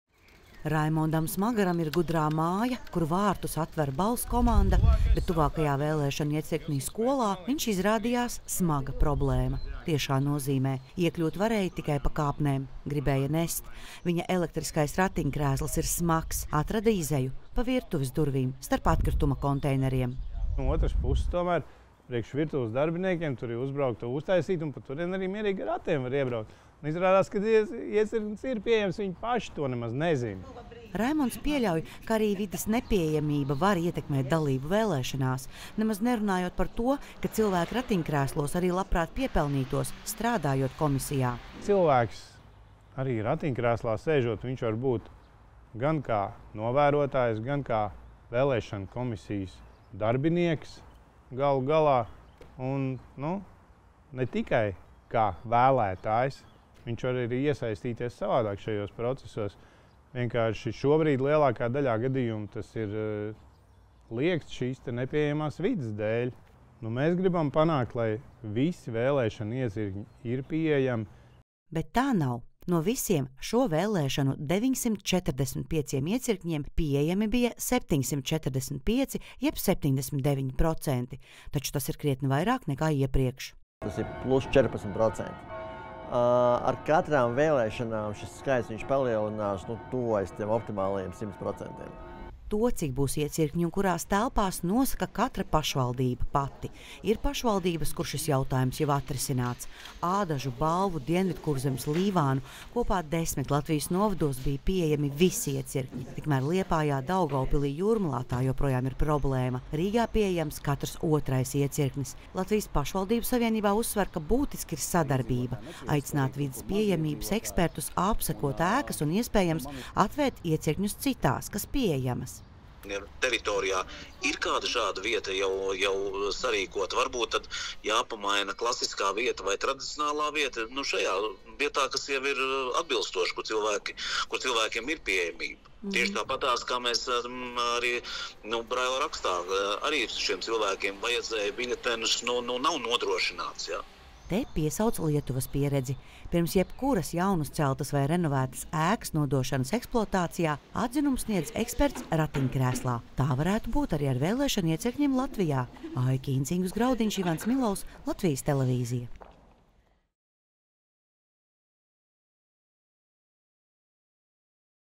Nepieejamie iecirkņi pazemo vēlētājus un liedz tajos strādāt
Šajās vēlēšanās no 945 vēlēšanu iecirkņiem nepieejami tiem, kam ir kādas pārvietošanās grūtības, bija 200 iecirkņi. Lielākā daļa atrodas lielajās pilsētās – Liepājā, Daugavpilī un Rīgā. Vides nepieejamība pazemo jaunos vecākus ar bērnu ratiem, cilvēkus ratiņkrēslos un seniorus ar kruķiem, kā arī arī liedz cilvēkiem ar invaliditāti papildus nopelnīt, strādājot vēlēšanu iecirknī.